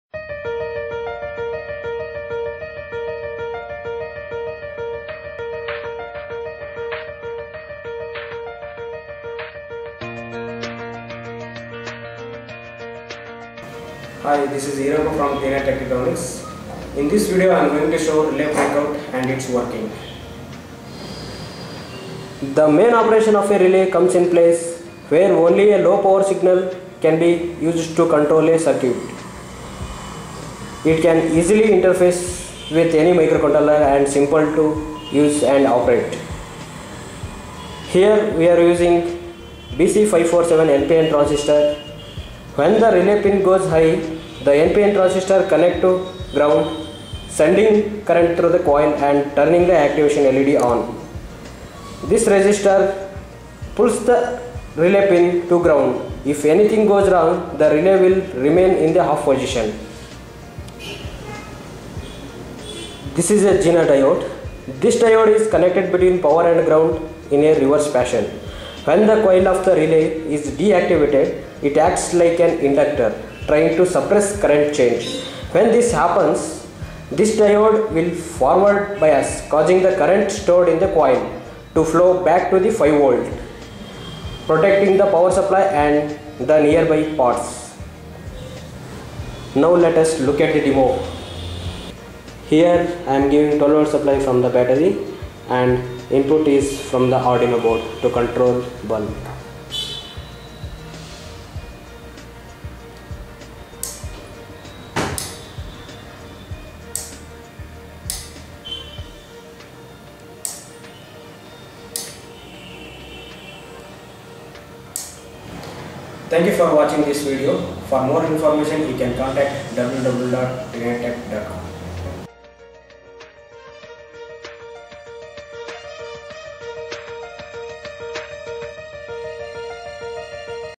Tenet Technetronics: Introducing Relay Breakout
Introducing Relay Breakout - Tenet Technetronics.
Relays are simple switches which are operated both electrically and mechanically. Relays consist of an electromagnet and also a set of contacts. The switching mechanism is carried out with the help of the electromagnet